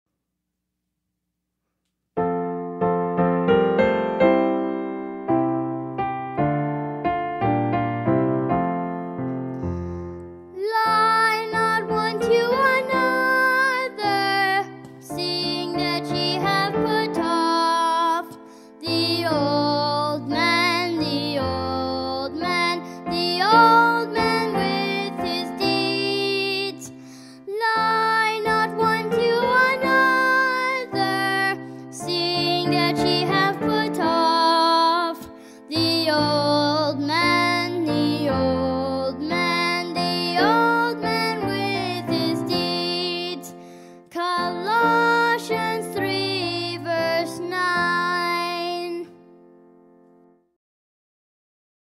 Colossians 3:9
Lie not one to another,
seeing that ye have put off
the old man with his deeds;